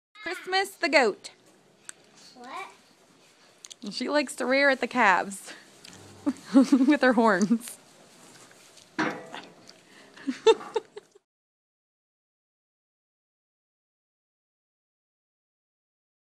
Goat rearing at the cows
We never knew goats could be this playful and intelligent 🤣🤓 So funny and cute! Don't you think so?